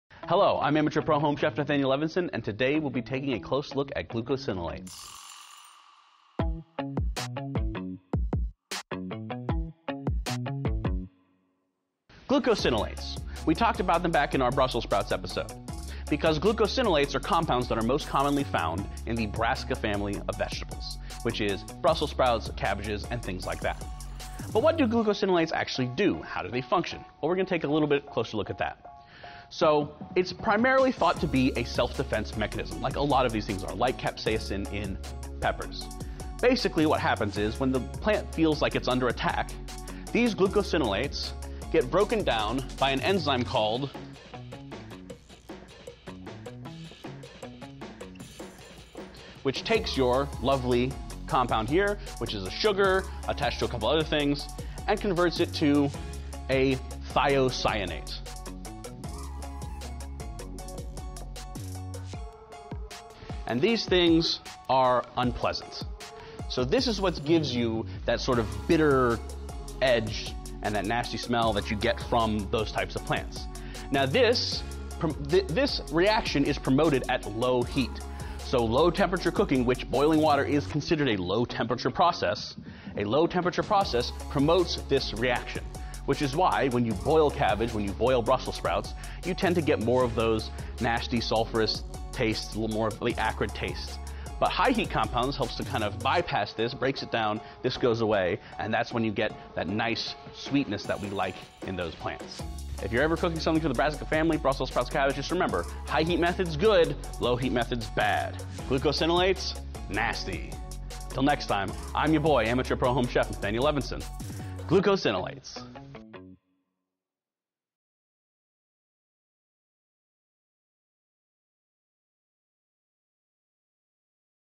Science-y Stuff - Glucosinolates
Amateur Pro Home Chef Nathanael Levinson learns ya about glucosinolates!